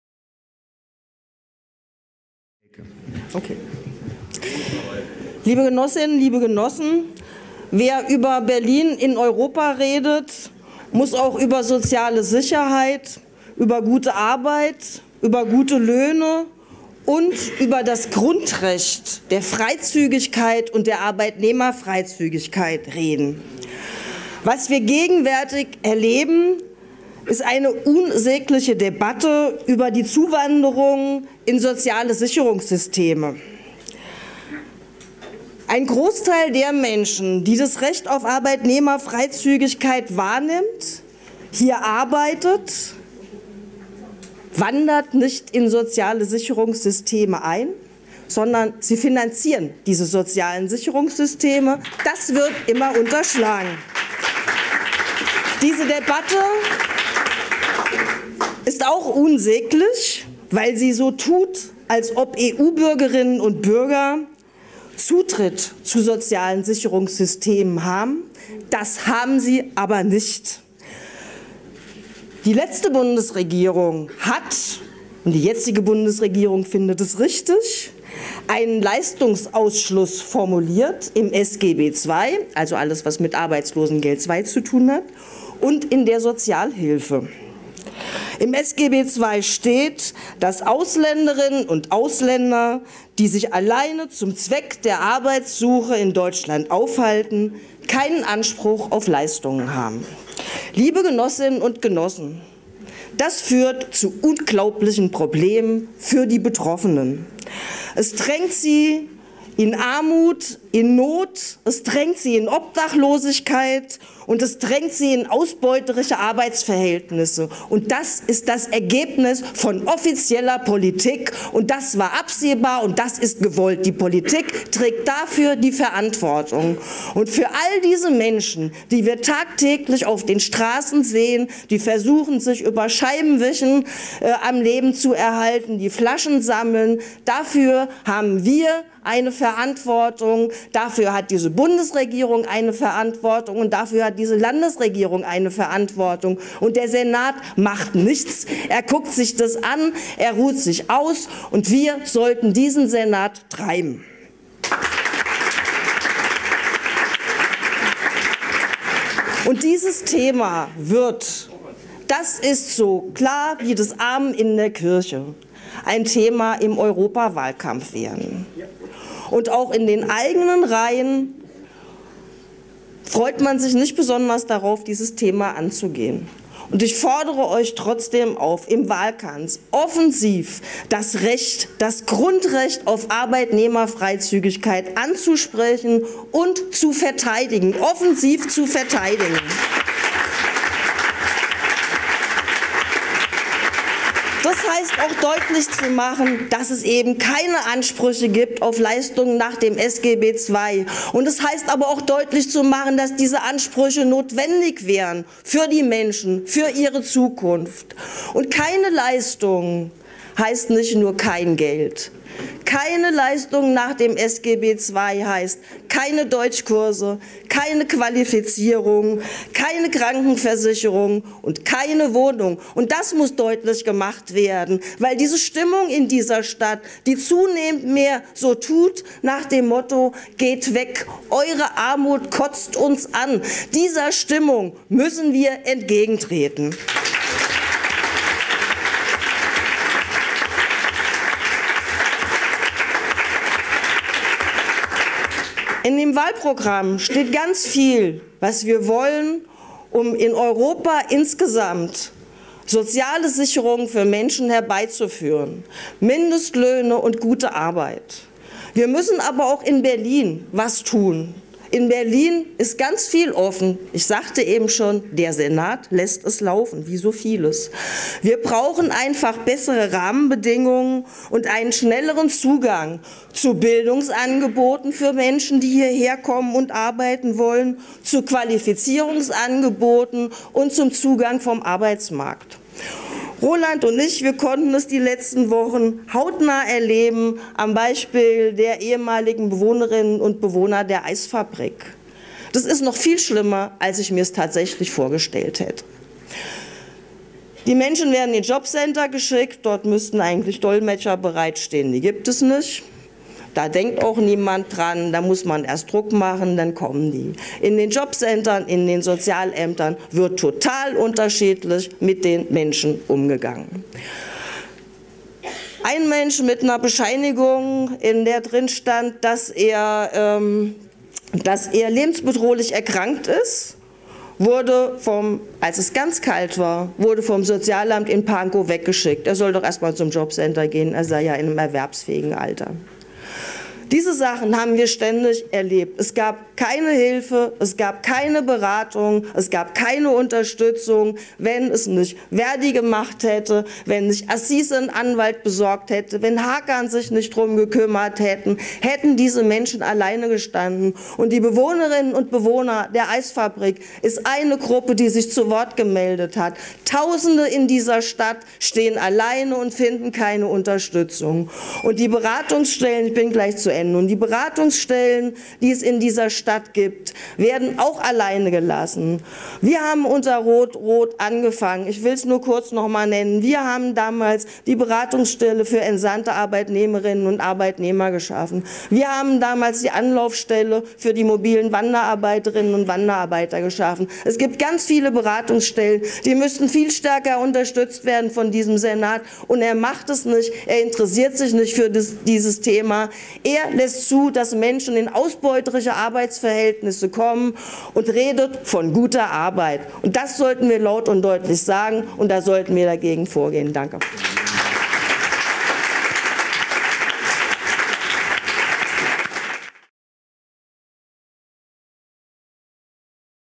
Elke Breitenbach zur Migrationspolitik in Berlin und Deutschland
(5. April 2014) Unsere stellvertretende Landesvorsitzende Elke Breitenbach über die verfehlte Flüchtlings- und Einwanderungspolitik von Bundesregierung und Berliner Senat.